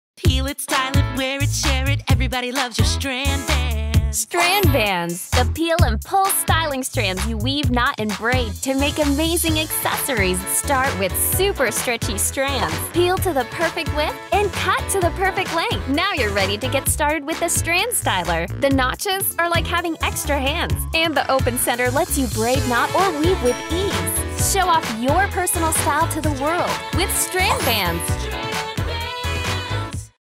Braid, Knot & Weave Fun Accessories with Strand Bands
With Strand Bands, you can create, show off, and share your own fun jewelry. Accessorize with unique designs and create your one of a kind Strand Bands bracelets, necklaces, and more.

Start with the super stretchy strands. Peel and cut them to the perfect length. Now you’re ready to get started with the Strand Styler. The Strand Styler holds the strands for you and the open center lets you braid, knot or weave with ease. Strand Bands slip on and off with ease, are waterproof, and can be undone and reused for new designs again and again. Mix and match beautiful beaded bracelets, fringed tassels for your bags, adorable rings for sweet street style, and add beads and charms for glitzy night glam.

Trade, mix and match your creations and share with friends. Show off your personal style to the world with Strand Bands.